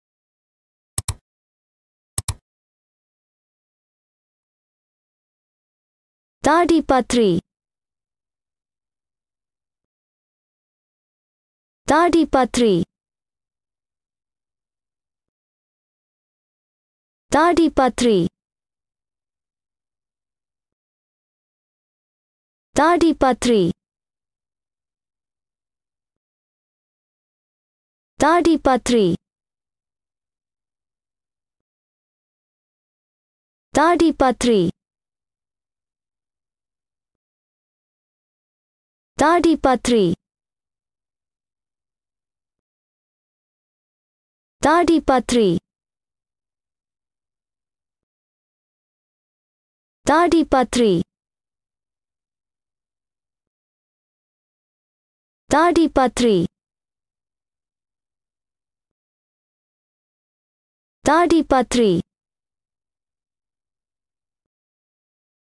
How to pronounce Tadipatri in English Correctly | Tadipatri a city in India
How to say Tadipatri Listen and learn the pronunciation of Tadipatri in English.

Tadipatri or Tadpatri is a city in Anantapur district of the Indian state of Andhra Pradesh at the border of Kurnool district and Kadapa district. It is the mandal headquarters of Tadipatri mandal in Anantapur revenue division. The Chintala Venkataramana Temple is located on a five-acre site in Tadipatri.